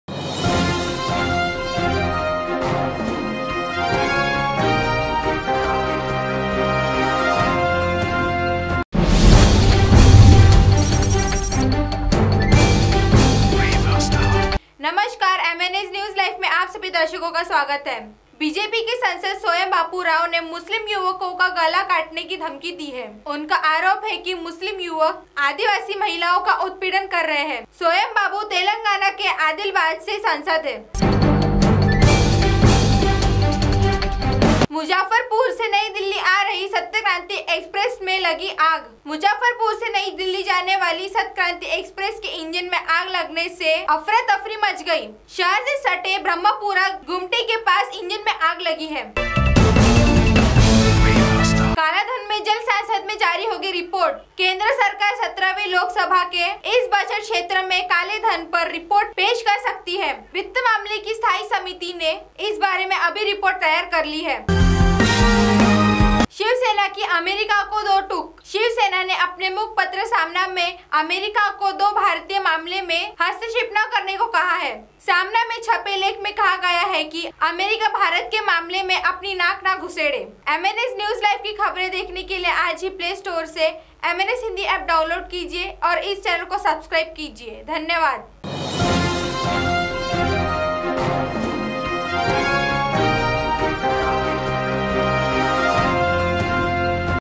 संसद ने मुस्लिम युवको के गला काटने की दी धमकी
बीजेपी संसद ने मुस्लिम युवको के गला काटने की दी धमकी MNSNEWSLIVE #